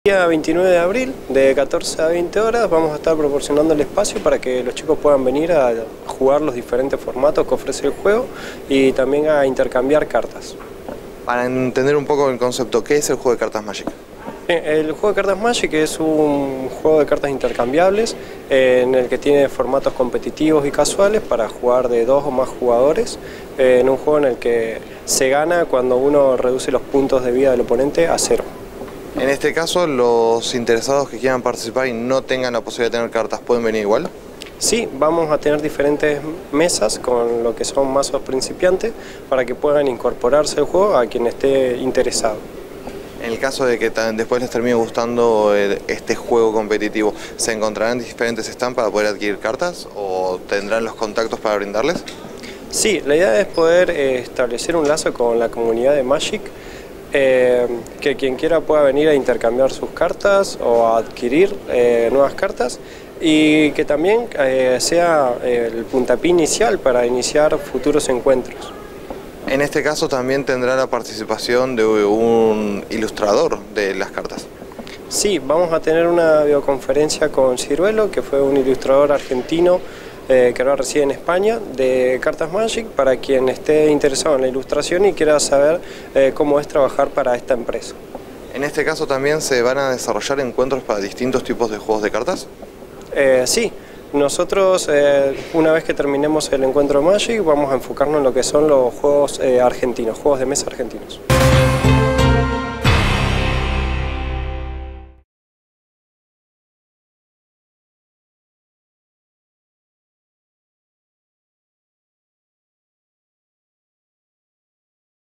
ENCUENTRO PROVINCIAL DE CARTAS MAGIC.
El 29/04 se llevará a cabo el evento de cartas apto para toda la familia de 14 a 20hs. en el Polo Creativo